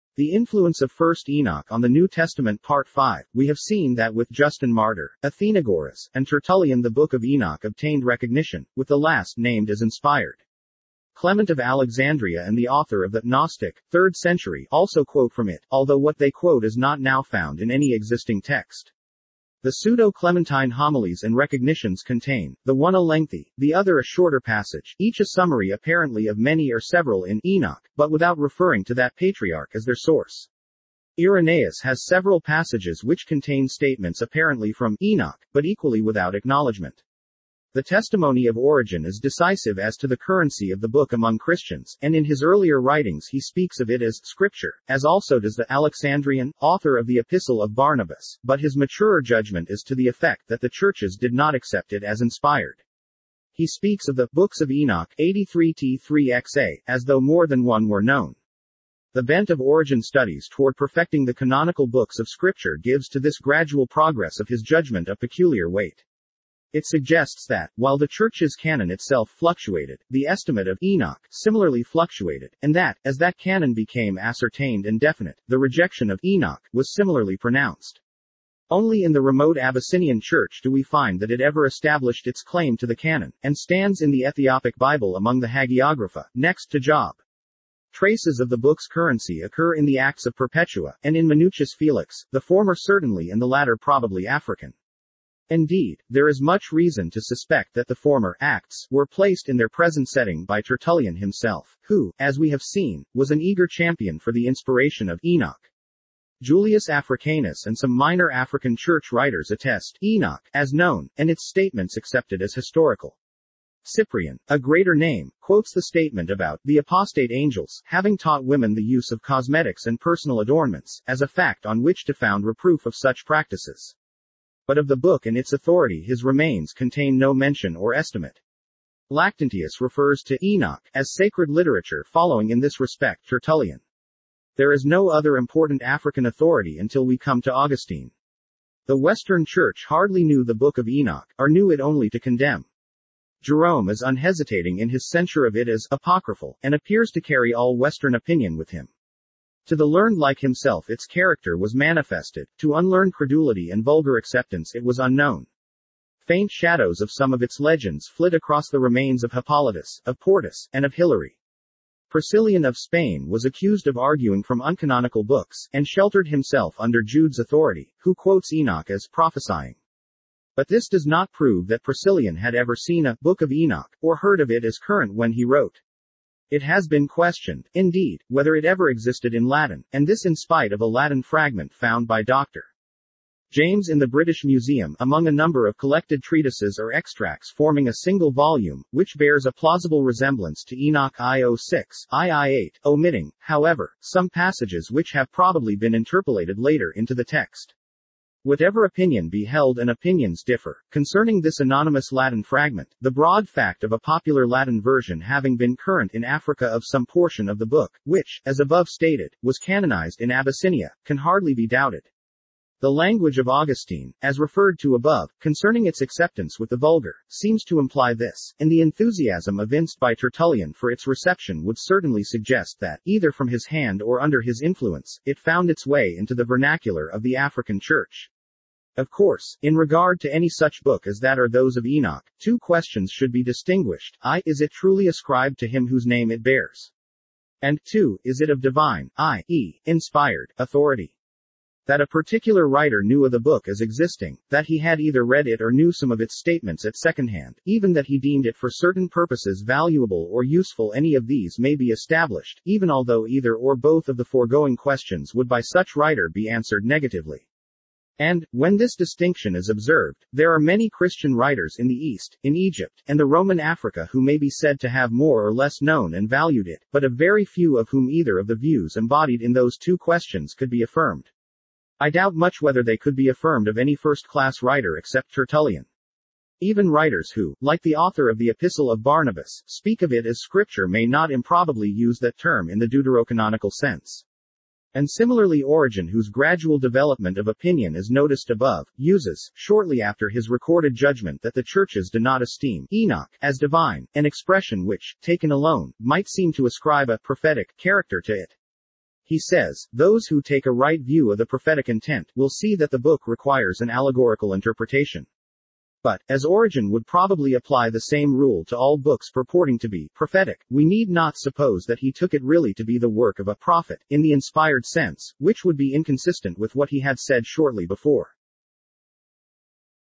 THE INFLUENCE OF 1 ENOCH ON THE NEW TESTAMENT PART V
We have seen that with Justin Martyr, Athenagoras, and Tertullian the book of Enoch obtained recognition, with the last named as inspired. Clement of Alexandria and the author of the (Gnostic, third century) Hlot'r toaua also quote from it, although what they quote is not now found in any existing text. The pseudo-Clementine Homilies and Recognitions contain, the one a lengthy, the other a shorter passage, each a summary apparently of many or several in "Enoch," but without referring to that patriarch as their source. Irenaeus has several passages which contain statements apparently from "Enoch," but equally without acknowledgment. The testimony of Origen is decisive as to the currency of the book among Christians, and in his earlier writings he speaks of it as " Scripture," as also does the (Alexandrian ?) author of the epistle of Barnabas; but his maturer judgment is to the effect that the churches did not accept it as inspired. He speaks of the "books of Enoch" (83ti3Xa), as though more than one were known. The bent of Origen's. studies toward perfecting the canonical books of gives to this gradual progress of his judgment a peculiar It suggests that, while the church's canon itself fluctuated, estimate of "Enoch" similarly fluctuated, and that, canon became ascertained and definite, the rejection was similarly pronounced. Only in the remote church do we find that it ever established its claim to and stands in the Ethiopic Bible among the hagiographa, to Job. Traces of the book's currency occur in the acts of Perpetua, and in Minucius Felix, the former certainly and the latter probably African. Indeed, there is much reason to suspect that the former "acts" were placed in their present setting by Tertullian himself, who, as we have seen, was an eager champion for the inspiration of "Enoch." Julius Africanus and some minor African church writers attest " Enoch " as known, and its statements accepted as historical. Cyprian, a greater name, quotes the statement about "the apostate angels" having taught women the use of cosmetics and personal adornments, as a fact on which to found reproof of such practices. But of the book and its authority his remains contain no mention or estimate. Lactantius refers to " Enoch " as Sacred Literature-following in this respect Tertullian. There is no other important African authority until we come to Augustine. In two important pas- sages, De Civ. Dei I5:23 and I8:28, he decides that the canonicity of " Enoch " is nil, that its statements are a mixture of some truth with much fiction, and its authority unverifiable, although popular credulity accepted it with promiscuous readiness. Hence, he says, it carries no weight "either among the Jews or ourselves." The western church hardly knew the book of Enoch, or knew it only to condemn. Jerome is unhesitating in his censure of it as "apocryphal," and appears to carry all western opinion with him. To the learned like himself its character was manifested; to unlearned credulity and vulgar acceptance it was unknown. Faint shadows of some of its legends flit across the remains of Hippolytus, of Portus, and  Of  Hilary. Priscillian of Spain was accused of arguing from uncanonical books, and sheltered himself under Jude's authority, who quotes Enoch as " prophesying." But this does not prove that Priscillian had ever seen a " book of Enoch," or heard of it as current when he wrote. It has been questioned, indeed, whether it ever existed in Latin, and this in spite of a Latin fragment found by Dr. James in the British Museum (among a number of collected treatises or extracts forming a single volume), which bears a plausible resemblance to Enoch Io6: I-I8, omitting, however, some passages which have probably been interpolated later into the text. Whatever opinion be held-and opinions differ - concerning this anonymous Latin fragment, the broad fact of a popular Latin version having been current in Africa of some portion of the book, which, as above stated, was canonized in Abyssinia, can hardly be doubted. The language of Augustine, as referred to above, concerning its acceptance with the vulgar, seems to imply this; and the enthusiasm evinced by Tertullian for its reception would certainly suggest that, either from his hand or under his influence, it found its way into the vernacular of the African church. Of course, in regard to any such book as that or those of Enoch, two questions should be distinguished: (I) Is it truly ascribed to him whose name it bears? and (2), Is it of divine, i. e., inspired, authority? That a particular writer knew of the book as existing, that he had either read it or knew some of its statements at second-hand, even that he deemed it for certain purposes valuable or useful -any of these may be established, even although either or both of the foregoing questions would by such writer be answered negatively.